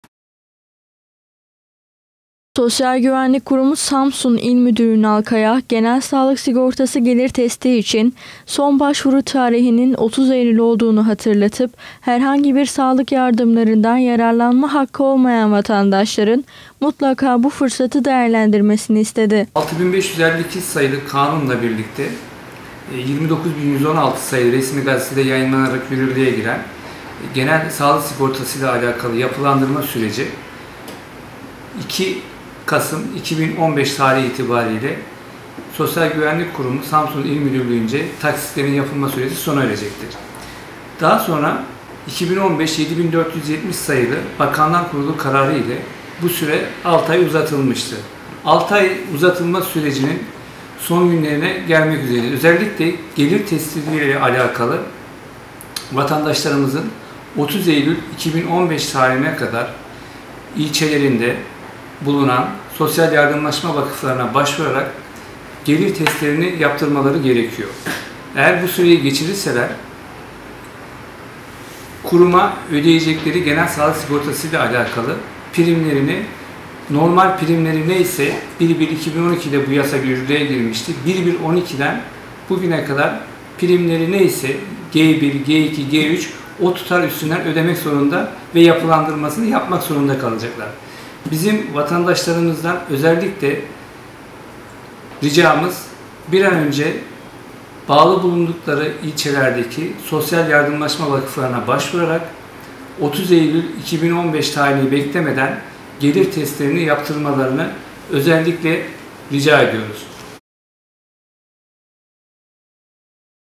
SGK MÜDÜRÜ KAYA'DAN ÇAĞRI
SGK Samsun İl Müdürü Ünal Kaya, Genel Sağlık Sigortası için gelir testinin 30 Eylül'e kadar mutlaka yaptırılması gerektiğini belirterek, ''30 Eylül GSS'de milattır. Vatandaşın bu güzel uygulanmadan faydalanmasını istiyoruz'' dedi.